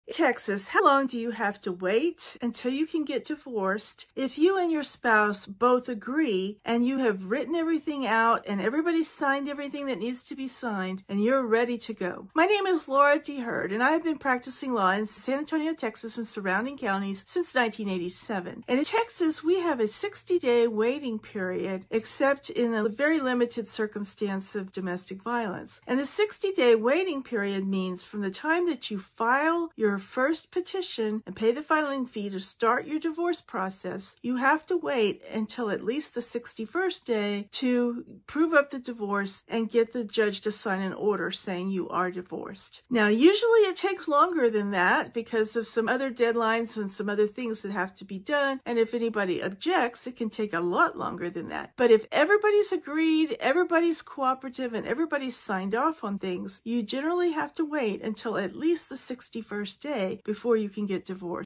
How Long Do You Have to Wait to Get Divorced in Texas?
Are you considering a divorce in Texas and wondering how long the process will take? In this video, Attorney Laura D. Heard, a seasoned family law expert with over 35 years of experience in San Antonio, Texas, explains the mandatory 60-day waiting period for divorce in Texas. Learn about the exceptions to this rule, including cases involving domestic violence, and understand what to expect during the divorce process.

Whether you're just starting the divorce process or are already in the midst of it, this video provides essential information to help you navigate your legal journey.

00:00 Introduction to Texas Divorce Waiting Period
00:18 Legal Requirements and Exceptions
00:42 Potential Delays and Final Steps